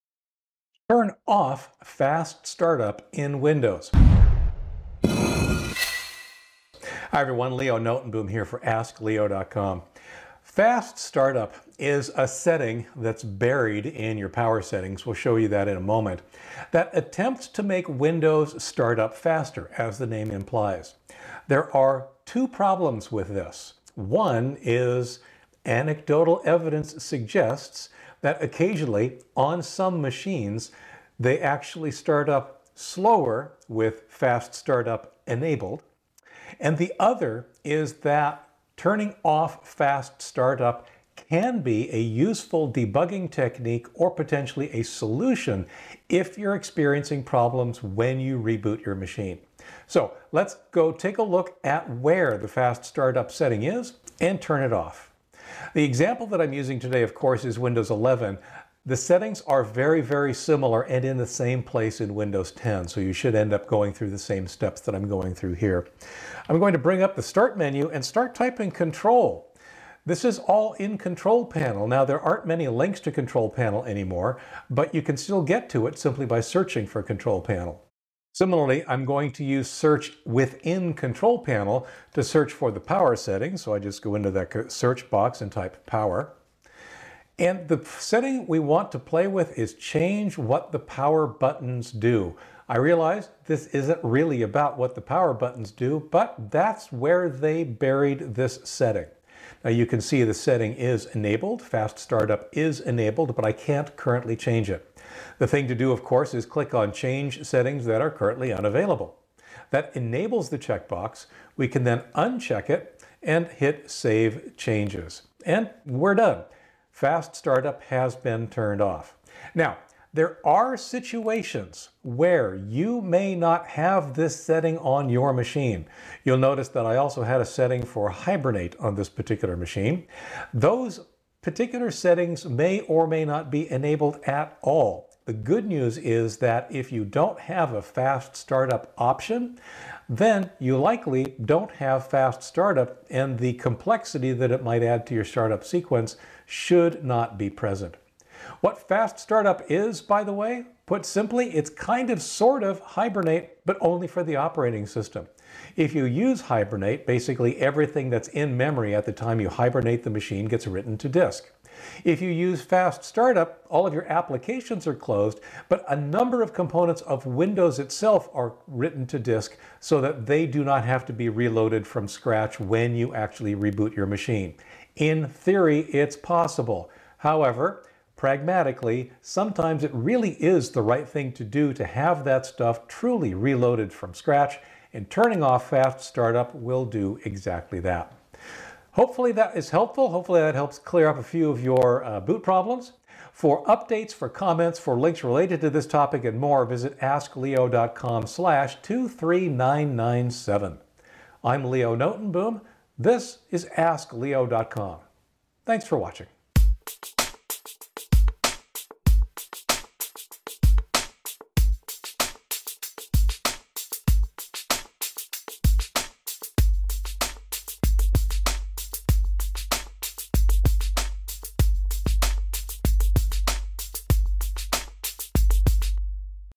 Turn Off Fast Startup in Windows
☀️ Fast startup optimizes boot-time speed, and is an occasional suspect in boot-time problems. Turn fast startup off to get clues.

☀️ Fast Startup
Fast startup has been implicated in some boot problems, and can, ironically, make some machines boot slower. The option to turn off fast startup is in Control Panel, under “Change what the power buttons do”.

Chapters
0:00 Turn Off Fast Startup
0:50 Looking at Fast Startup
2:00 What if the setting is not there?
2:30 What is Fast Startup?